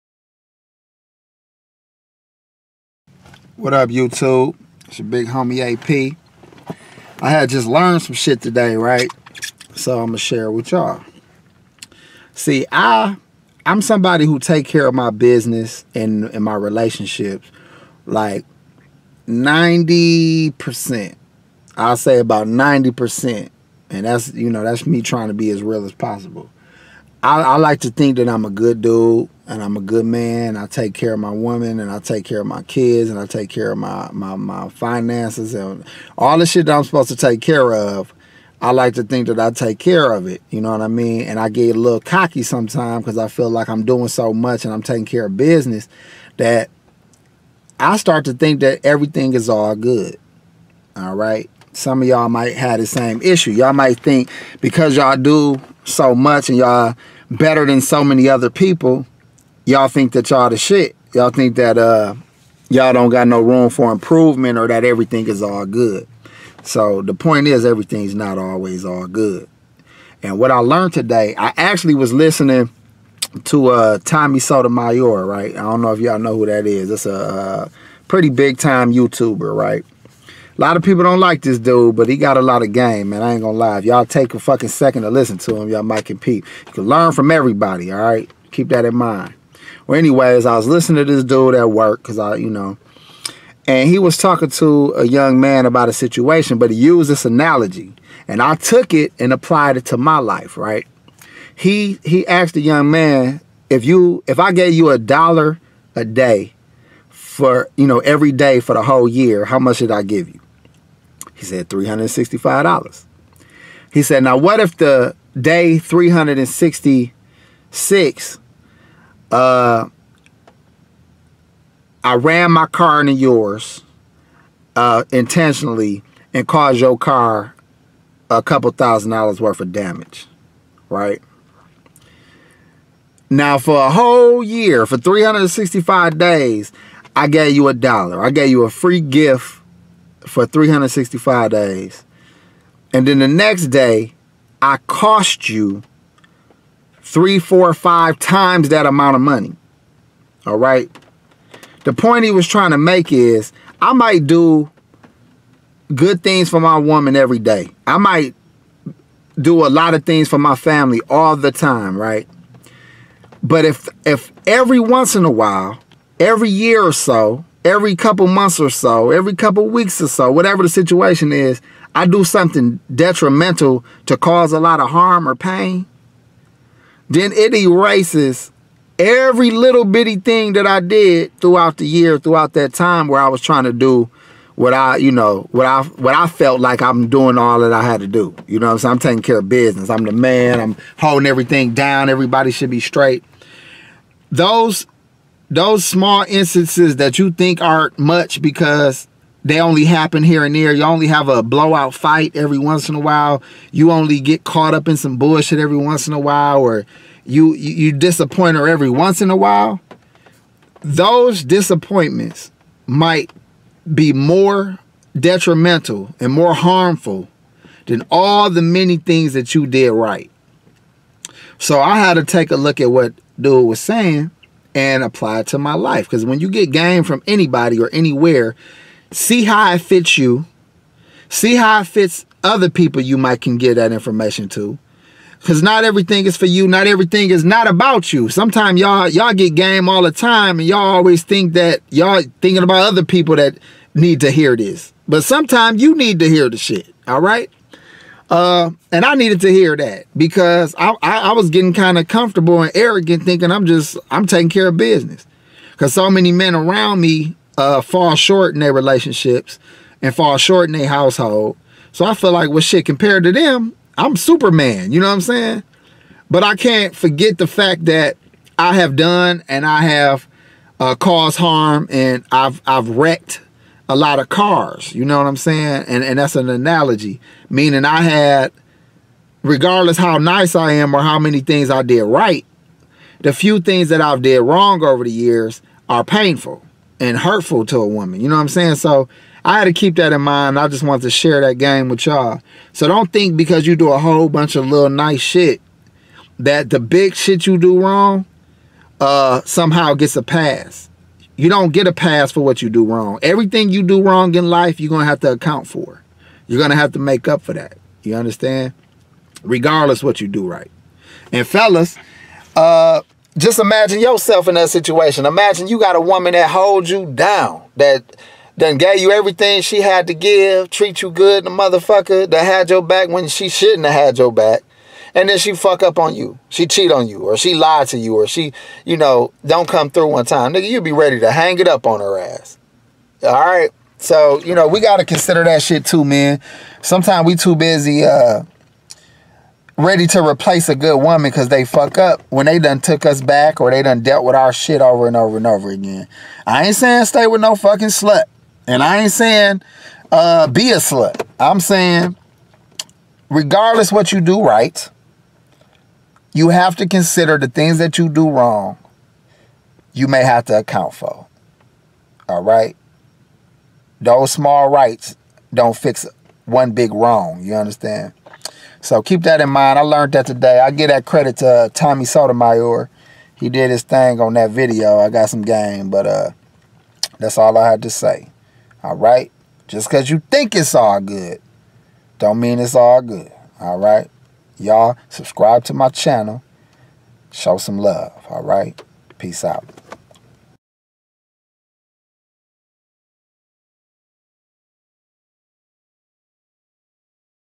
1 wrong erases 100 rights game from Tommy Sotomayor
You Tuber Tommy Sotomayor made a great point on one of his videos  that I had to apply to myself. Game can come from anywhere and anyone so be quick to hear and slow to speak.